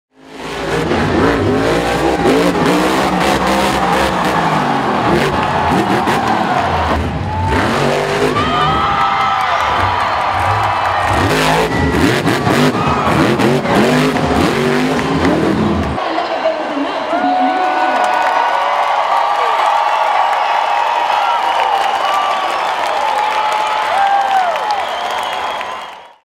Todd LeDuc | Monster Energy Freestyle & HUGE air │ Monster Jam World Finals XX (20)
Todd LeDuc's awesome Freestyle with HUGE air during Saturday's Monster Jam World Finals XX (20) Freestyle Competition inside Camping World Stadium in Orlando, Florida.